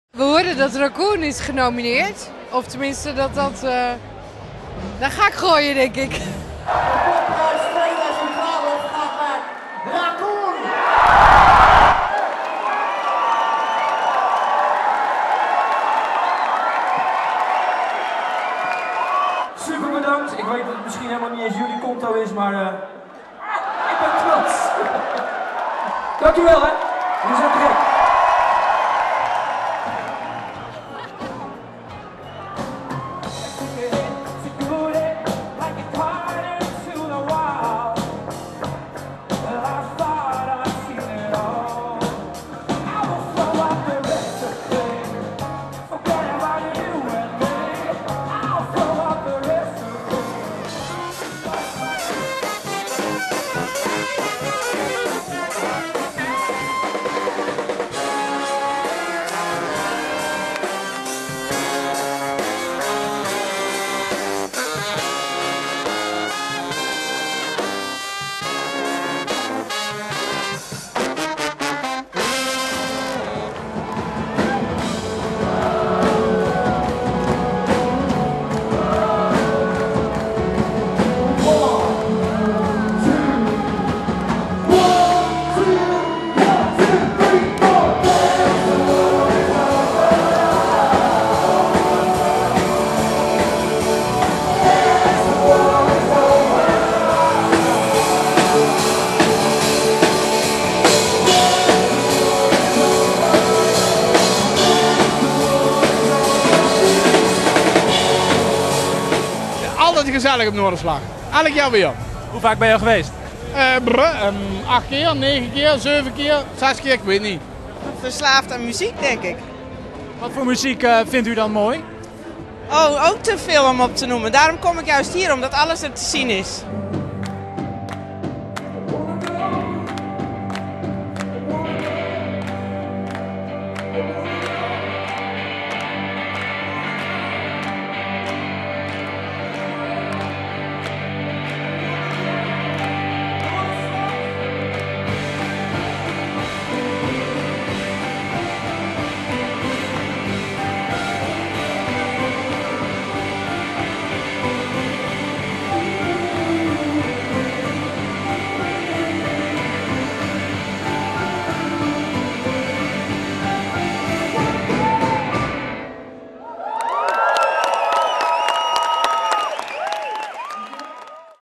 Racoon wint Popprijs 2012 - RTV Noord
Tijdens Noorderslag 2013 in de Oosterpoort in Groningen heeft de band Racoon de Popprijs 2012 gewonnen...